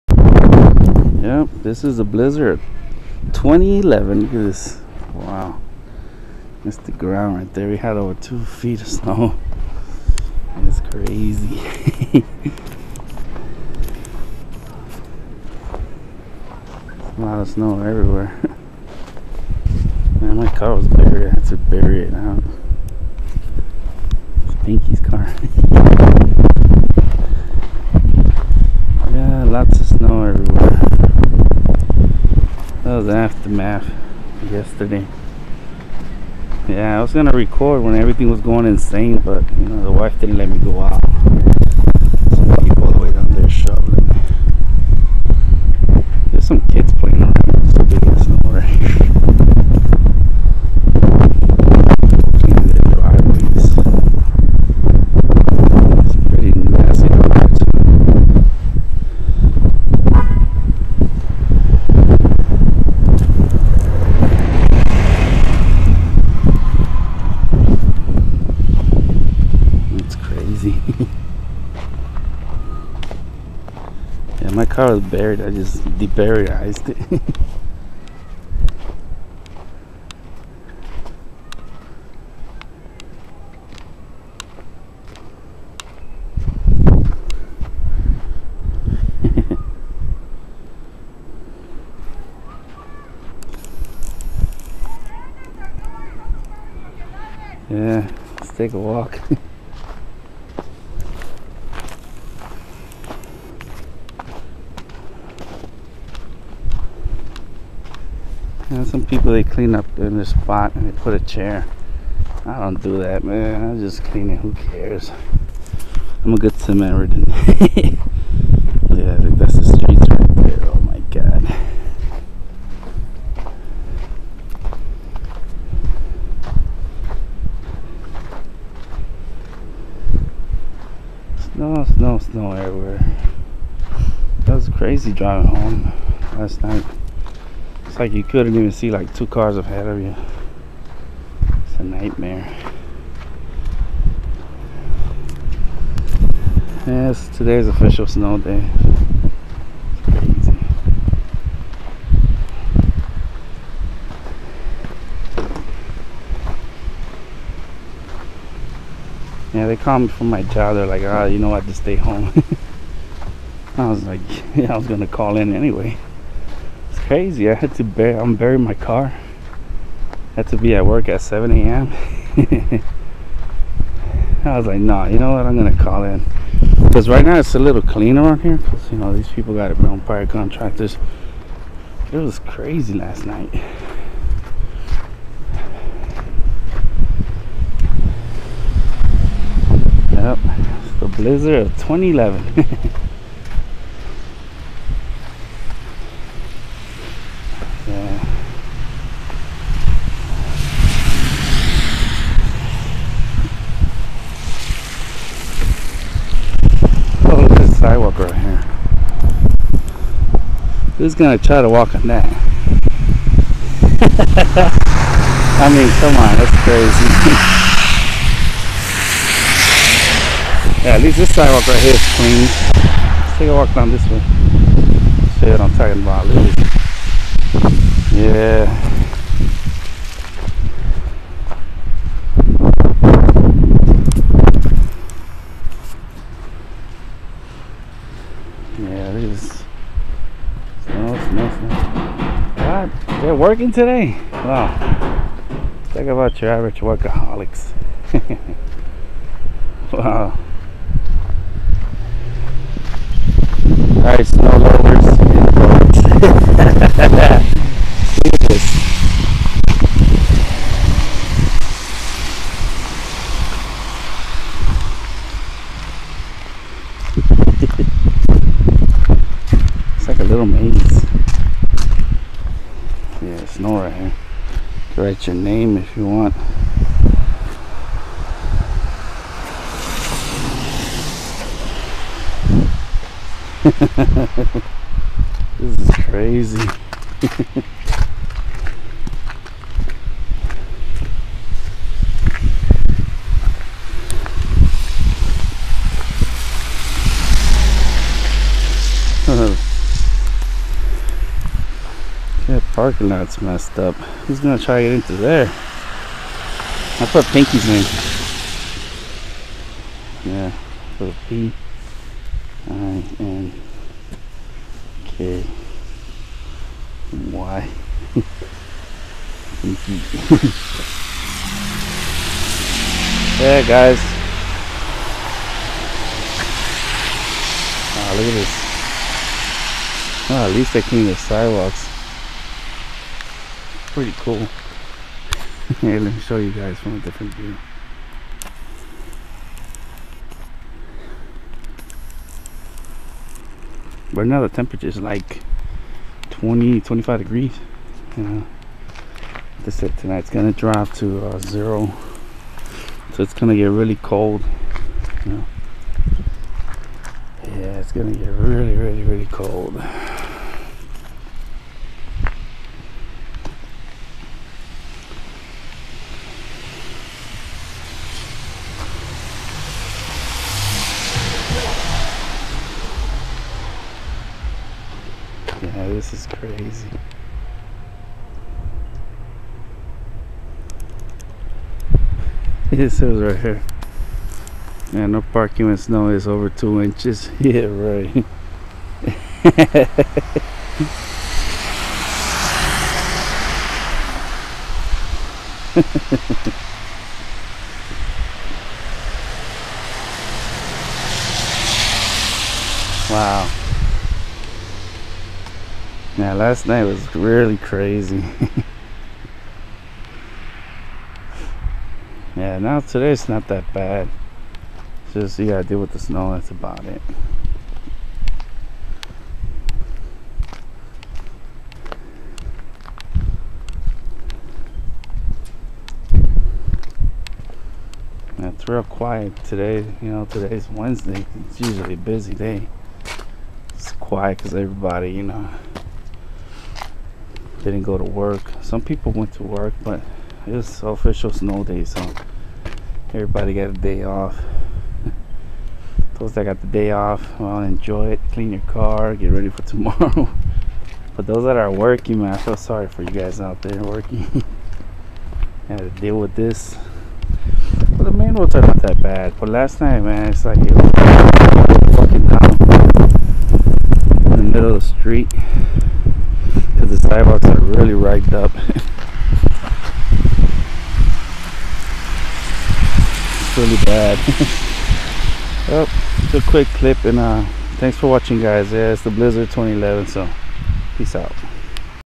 CHICAGO BLIZZARD 2011 (aftermath)
it was pretty bad last night drivin condition were very dangerous this is the after math